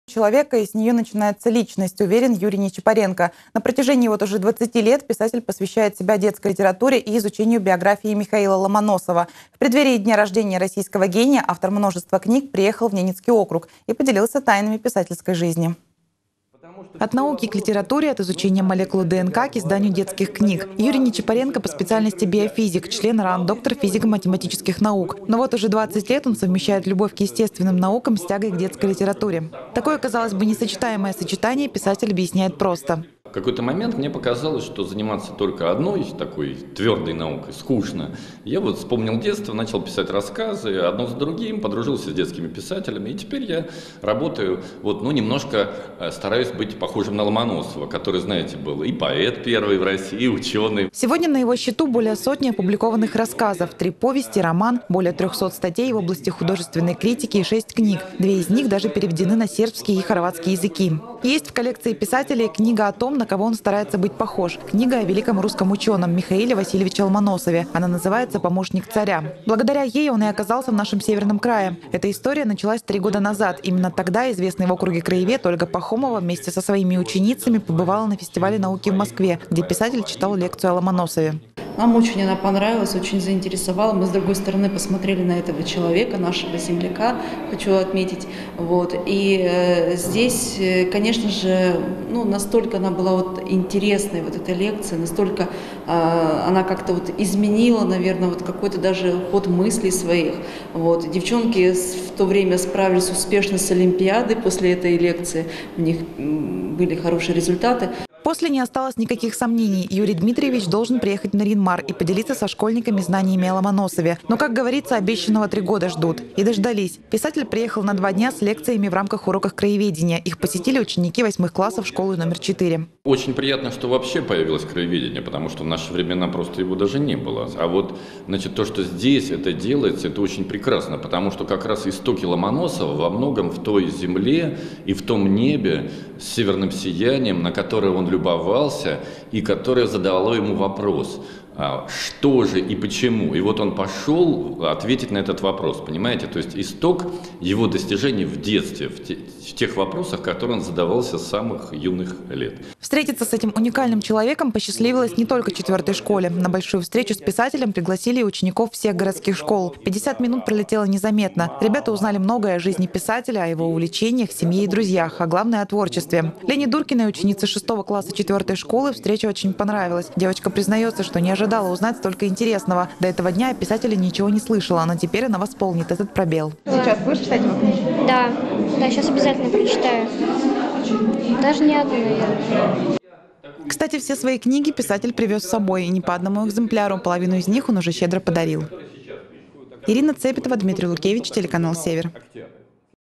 С лекциями о Ломоносове в НАО приехал Юрий Нечипоренко
Книга определяет судьбу человека, с нее начинается личность, уверен Юрий Нечипоренко. На протяжении вот уже 20 лет писатель посвящает себя детской литературе и изучению биографии Михаила Ломоносова. В преддверии дня рождения российского гения автор множества книг приехал в Ненецкий округ и поделился тайнами его жизни.